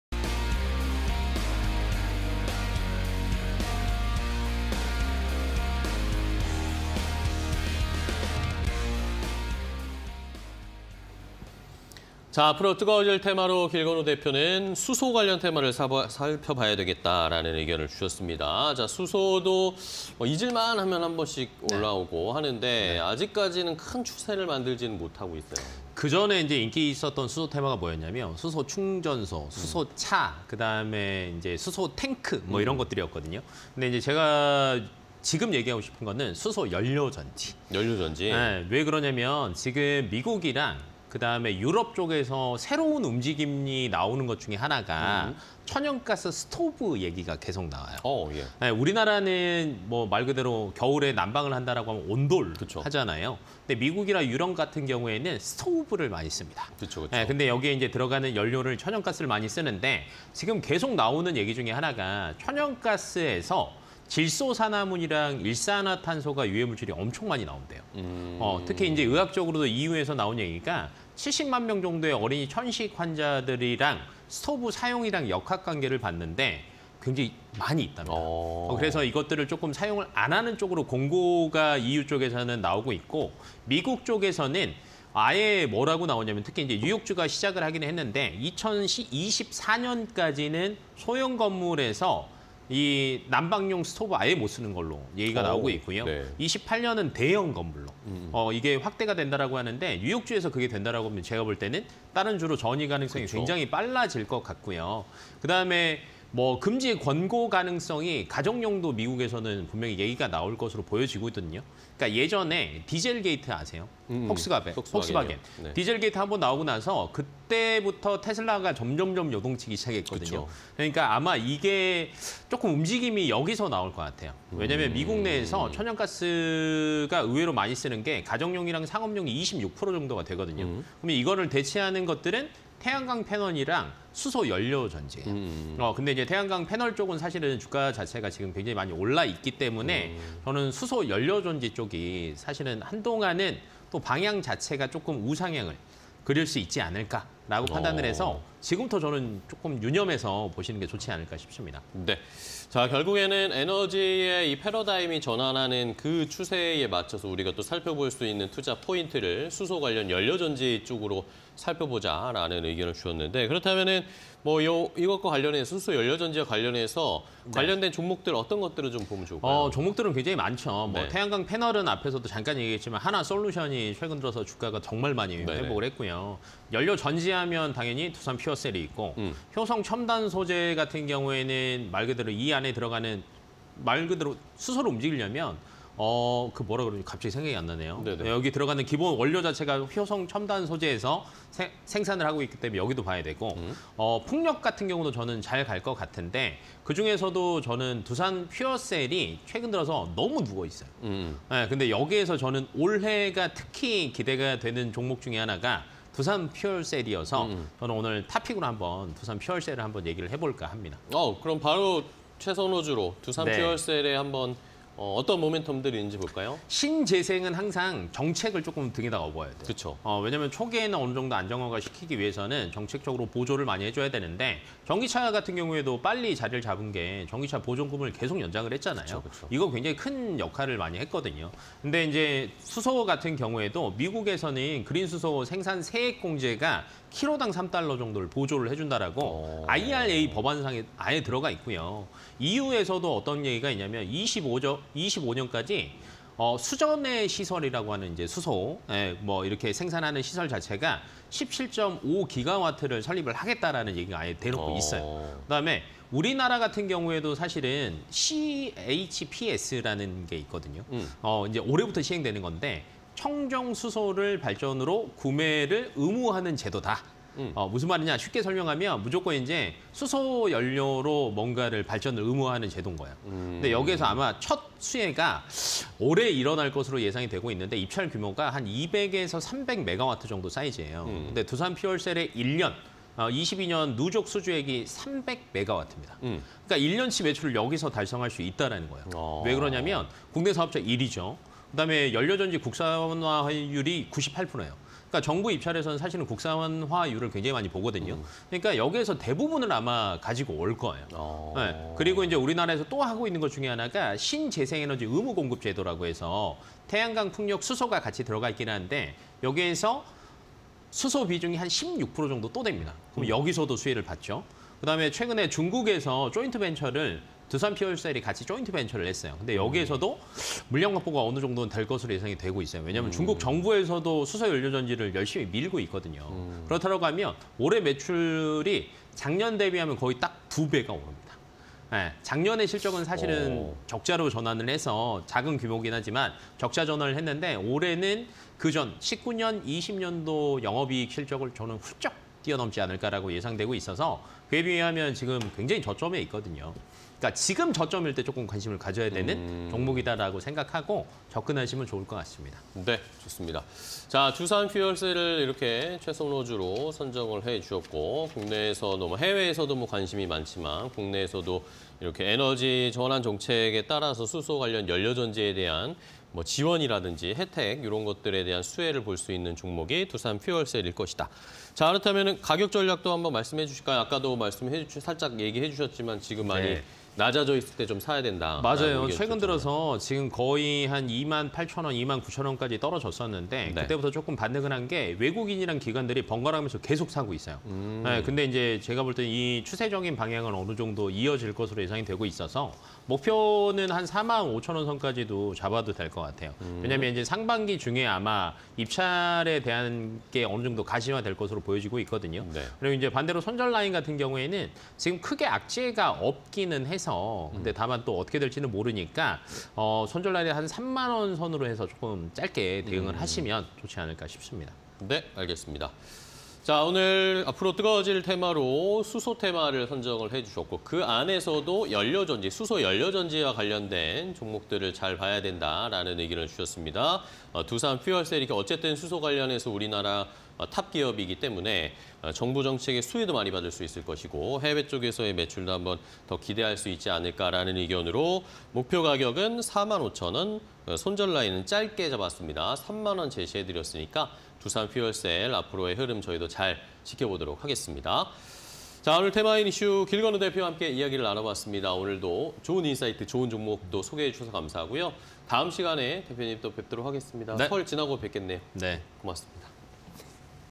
두산퓨얼셀(336260)... 수소 관련주_테마IN이슈 (20230116)
수소 관련주
- 美ㆍEU, '천연가스 사용 감소 정책' 계획
- 가정 내 천연가스 스토브, 유해 물질 배출
- 태양광 패널 등 가정ㆍ건물 천연가스 사용 대체
- 한화솔루션, 두산퓨얼셀, 효성첨단소재, 씨에스윈드, 삼강엠앤티 등

두산퓨얼셀(336260)
- 미국 IRA 및 EU 수전해 시설 설립 목표 등 수혜
- 지난해 수주 목표치 초과 및 CHPS 올해 도입 수혜
- 중국 파트너와 설립한 합작법인, 추가 물량 확보
- 2023년 매출 전년대비 2배 증가… 흑자전환 가능성